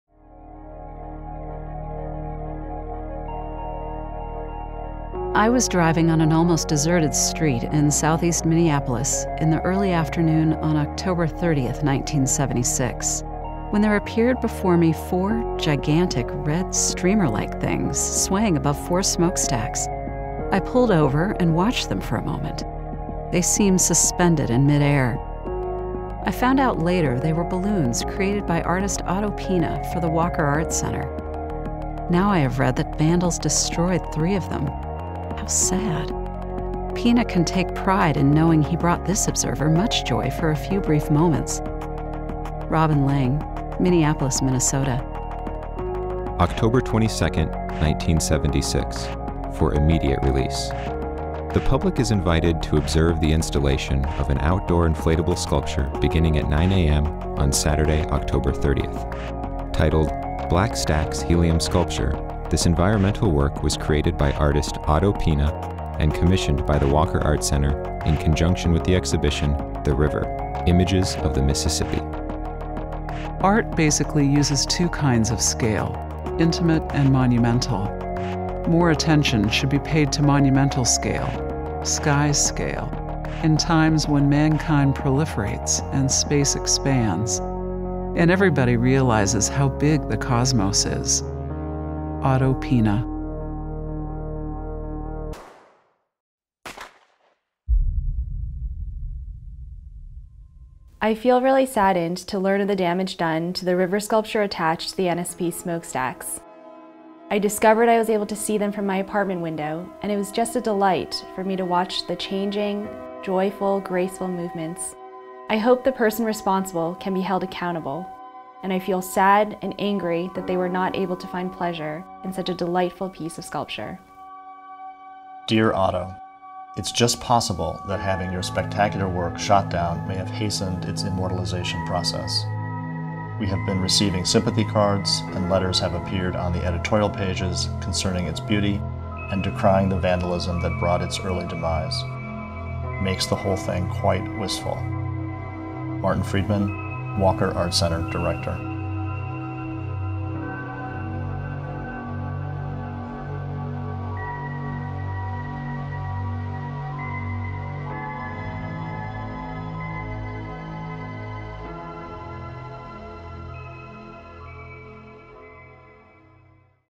The Rise & Demise of Otto Piene’s Black Stacks Helium Sculpture (1976)
"Dear Otto," wrote former Walker director Martin Friedman to German artist Otto Piene 40 years ago this week. "It's just possible that having your spectacular work shot down may have hastened its immortalization process." On Halloween of 1976, a vandal with a gun took down Piene's Walker-commissioned artwork Black Stacks Helium Sculpture, which consisted of four 300-foot-long, undulating inflatable tubes ascending from smokestacks on the Minneapolis riverfront. The work of "Sky Art," as Piene called it, was commissioned as part of the Walker exhibition The River: Images of the Mississippi and was intended to be on view for two weeks.